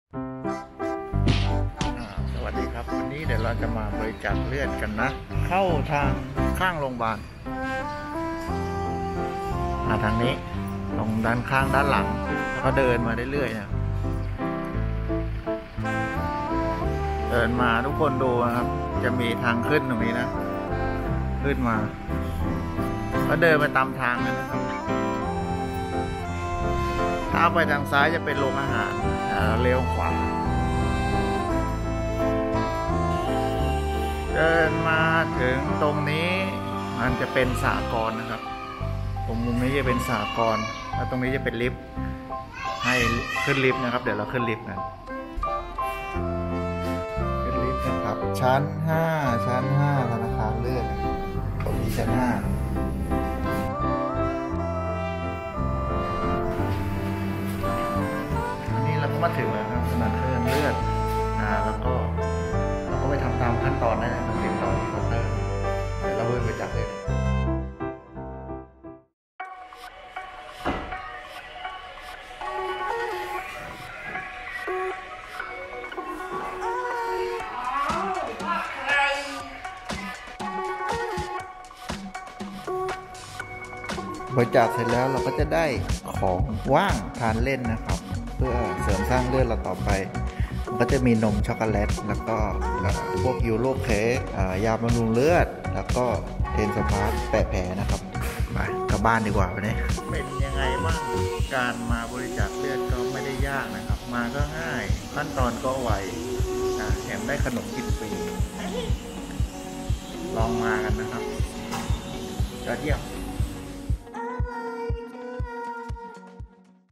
[4K] บริจาคเลือดกันที่โรงพยาบาลยะลา จังหวัดยะลา
ไปยังไง ตึกไหน ชั้นไหน เรามีคำตอบ
สงสัยอะไรแจ้งมาได้เลยนะ